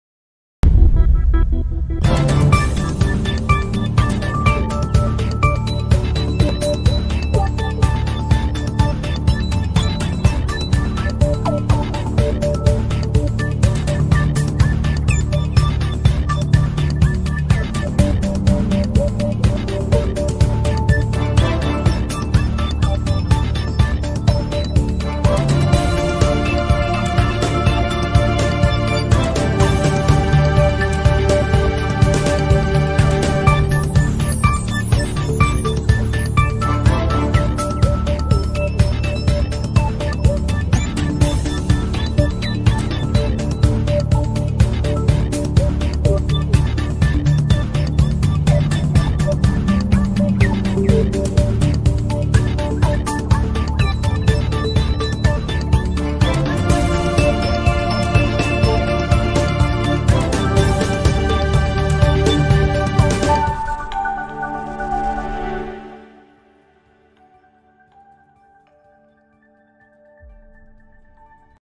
الوزارة الأولى: استئناف الأنشطة التجارية والخدماتية في كل الولايات
الجزائر (وأج) - قررت الحكومة استئناف مجموعة من الأنشطة التجارية والخدماتية في كل الولايات، وهذا ضمن مخطط المرحلة الثانية للخروج من الحجر بصفة تدريجية.